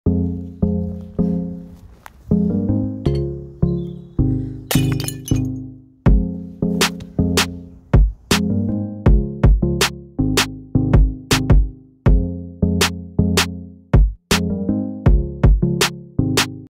Gonna Smash It - Teapot
It's Simple, i smash things.

Today i am going to smash a Teapot.